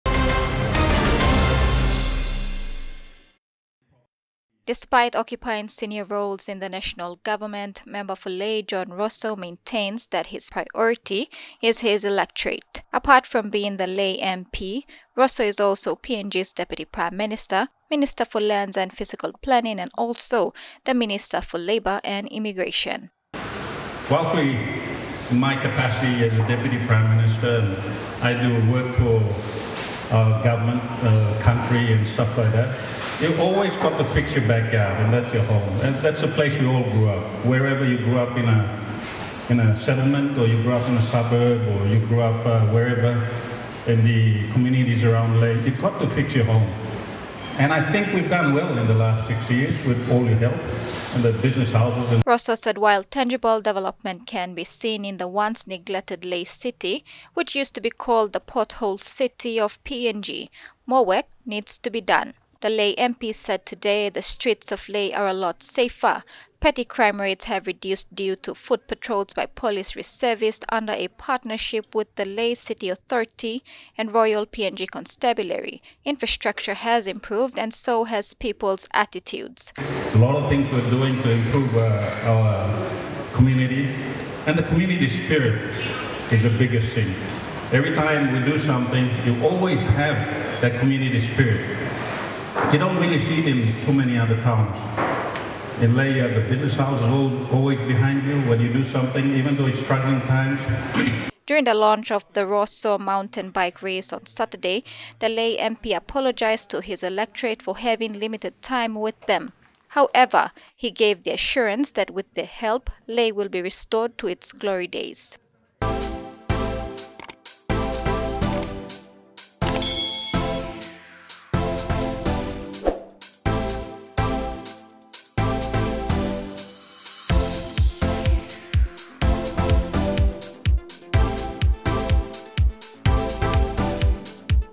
Fix Your Home
Despite holding onto senior portfolios in the National Parliament, the Member for Lae, John Rosso, stressed on the importance of fixing one's home.

Apart from being the Lae MP, Rosso is also PNG's Deputy Prime Minister, Minister for Lands and Physical Planning and the Minister for Labour and Immigration.